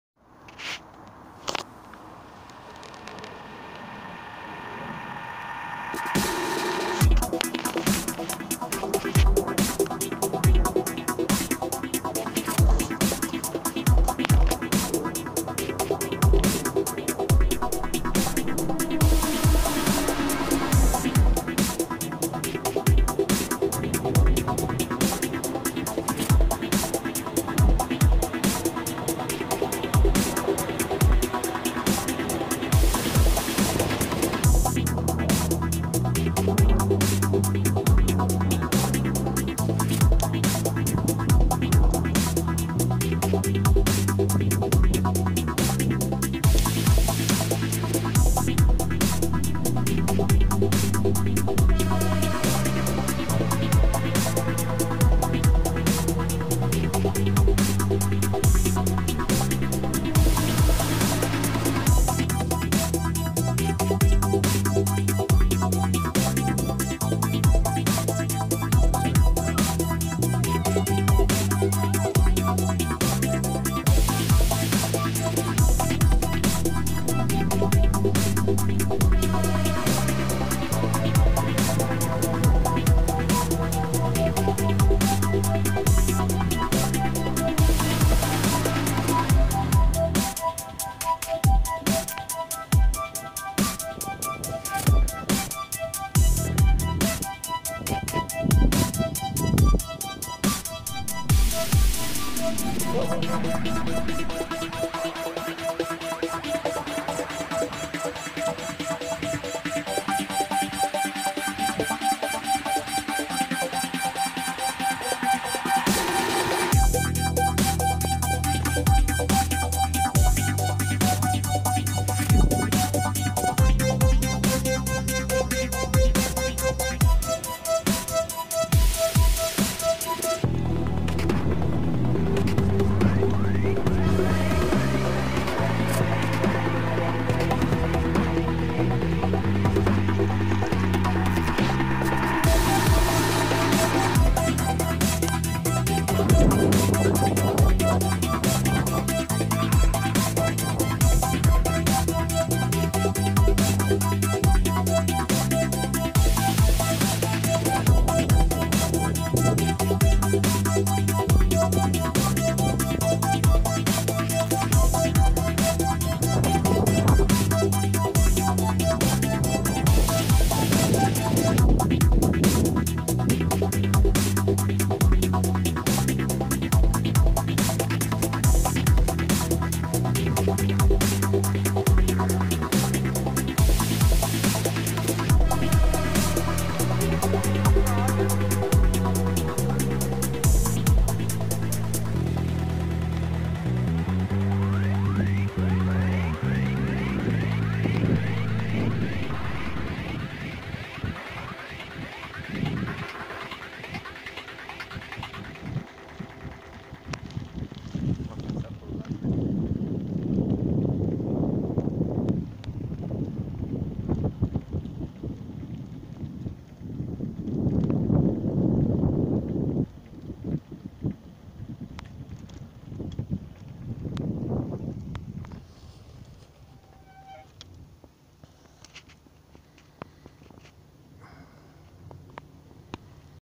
Riding around Virginia Water lake🌲
Riding around Virginia Water lake... lot's of pedestrians in the way!!!